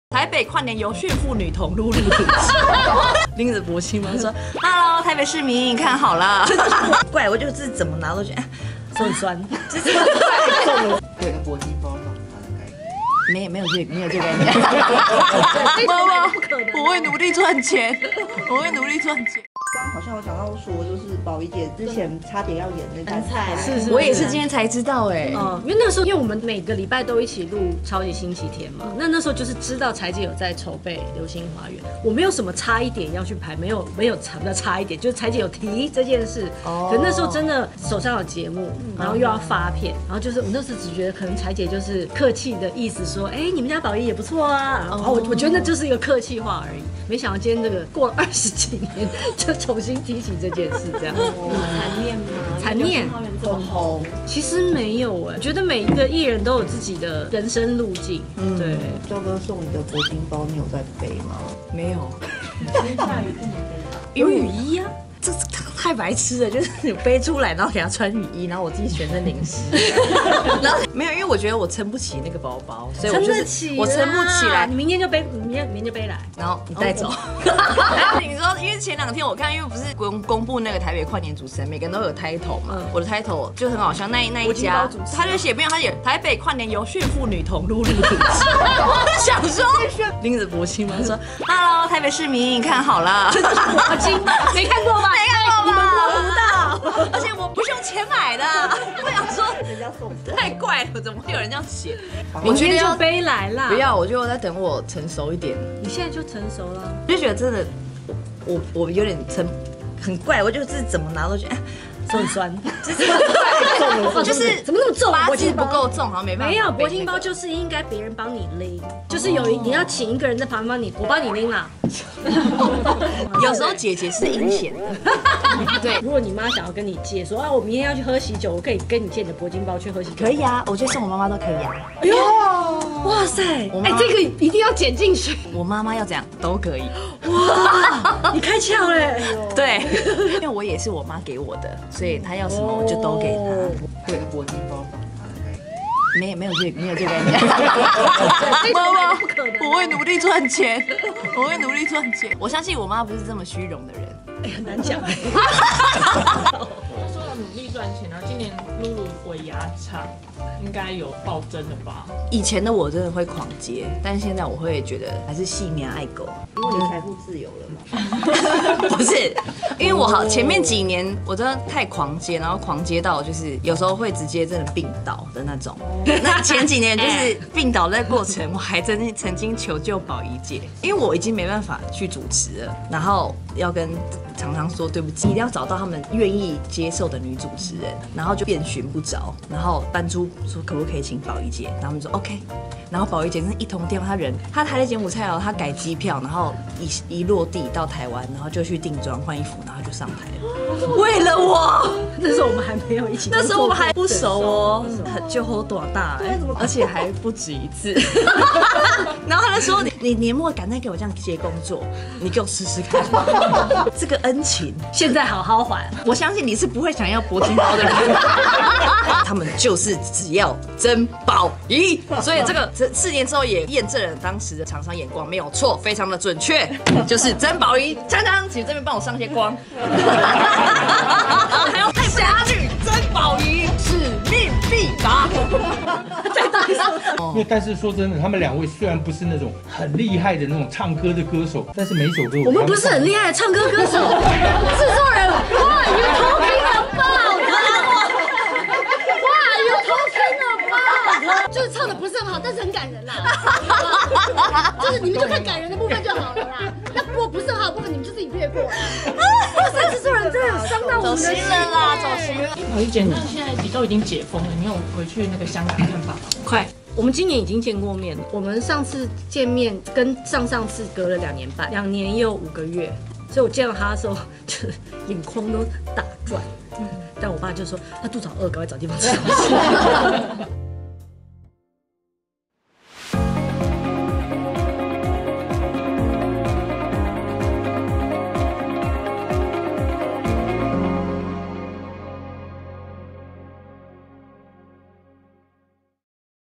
【星聞爆爆】Lulu病倒！曾寶儀出手救命2次 曝不敢拿「柏金包」送媽媽都沒關係
2013年TVBS歡樂台第二部原創概念劇